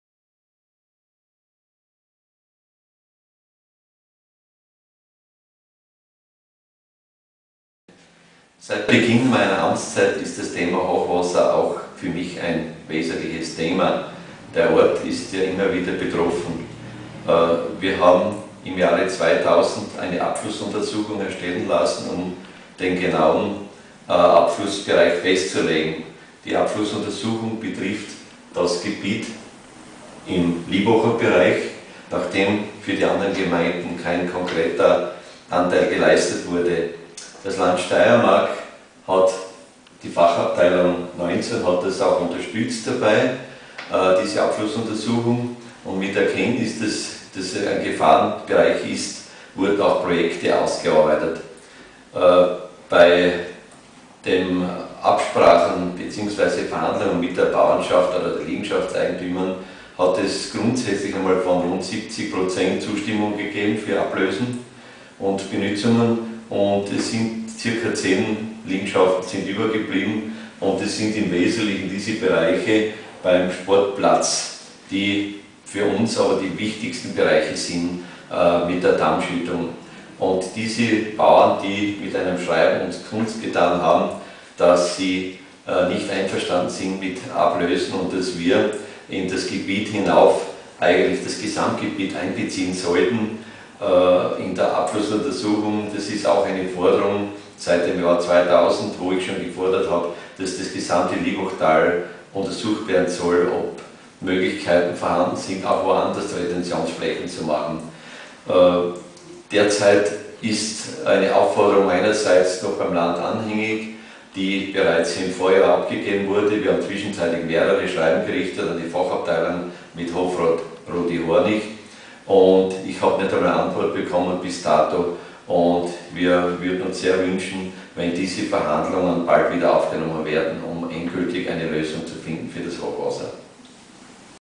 Hochwasserschutz in Lieboch September 2009
Lieboch Altbürgermeister Pignitter über den geplanten Hochwasserschutz - angeblich sind nur mehr 11 Grundstücke fraglich - es sollte jetzt schnell gehen.
Es werden aber weitere 5 Jahre vergehen, bis endlich Bewegung in die Sache kommt!